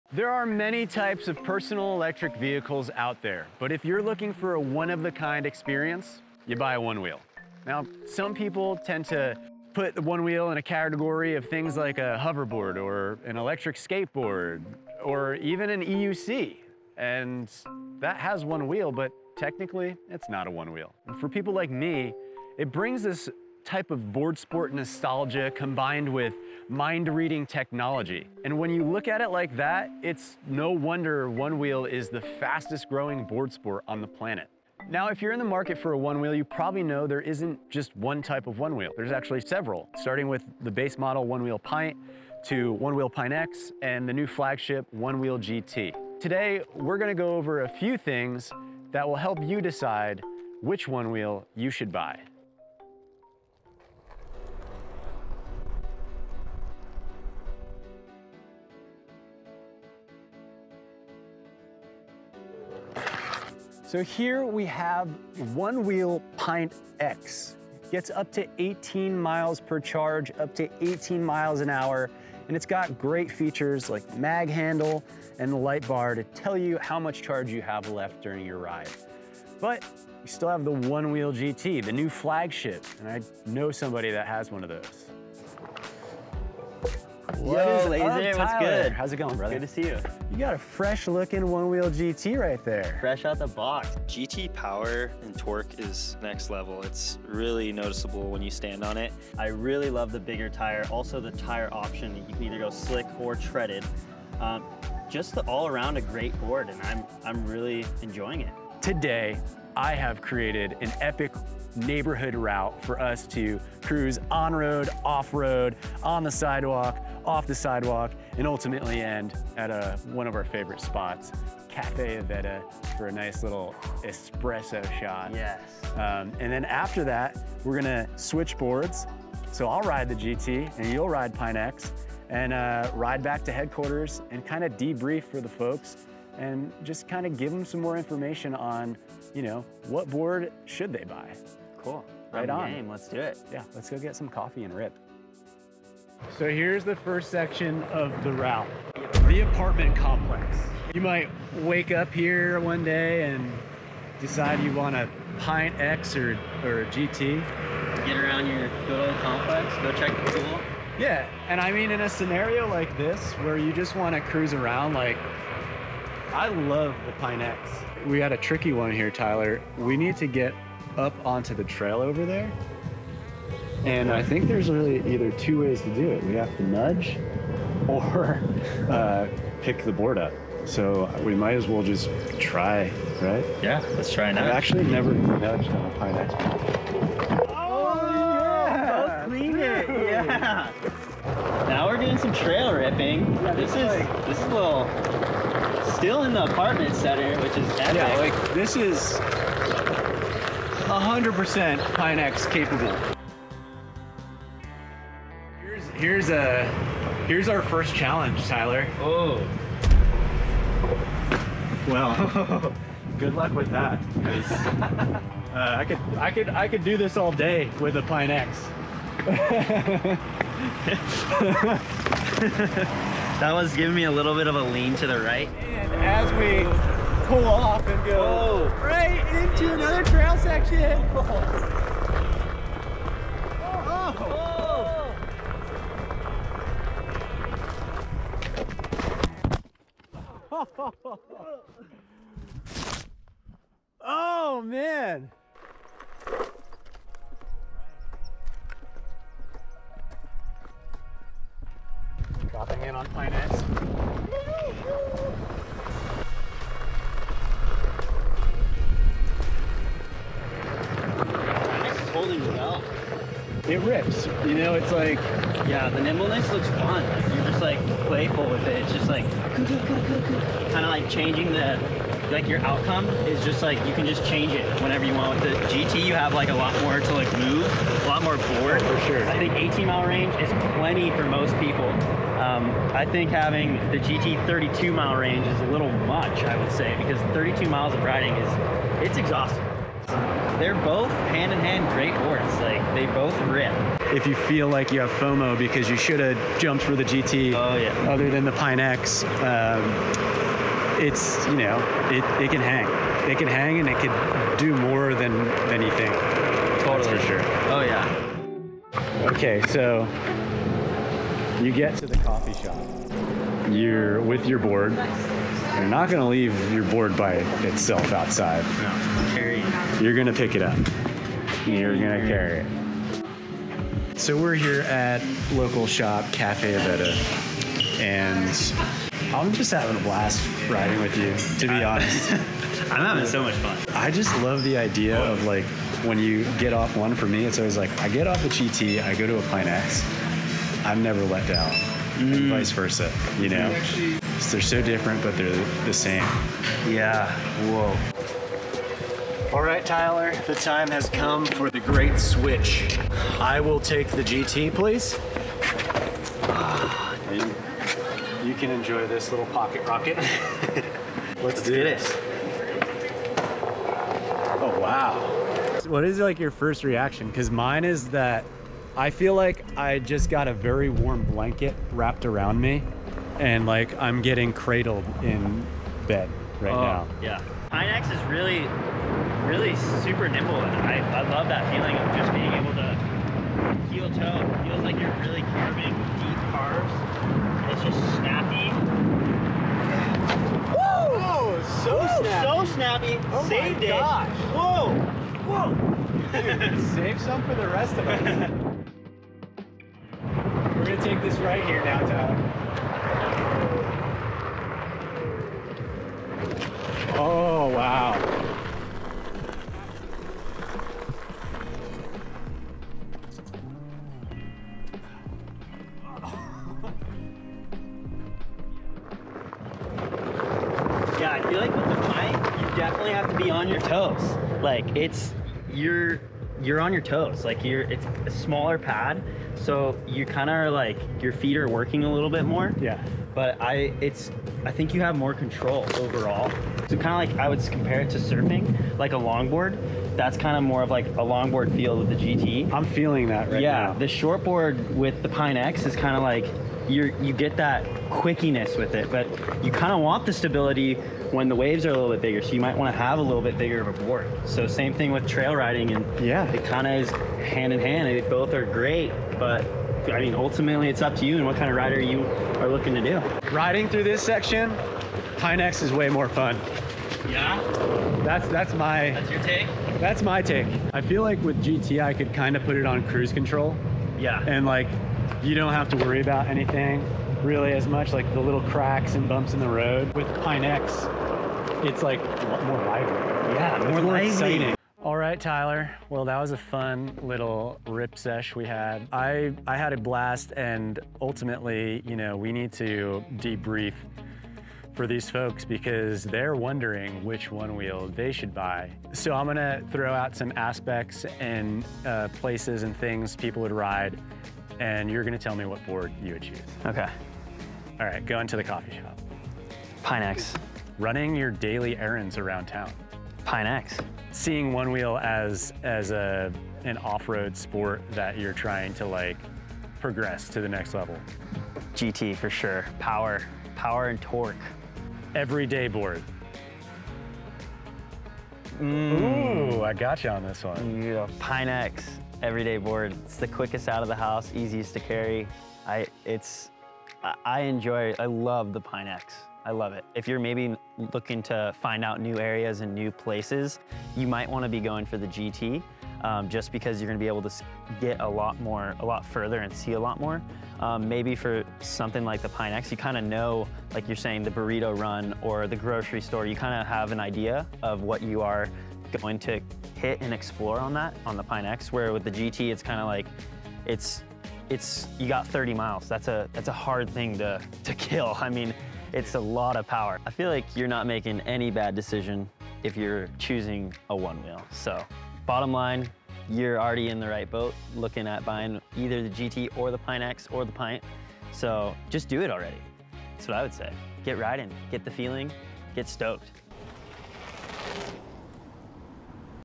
Onewheel GT vs. Onewheel Pint X | Which Onewheel Should You Get?
Thinking about getting a Onewheel but not sure which one is best suited for you? Adam and Tyler take Onewheel GT and Pint X for a coffee run and breakdown the key similarities and differences between the two boards. You can't go wrong on Onewheel so you're in the right place!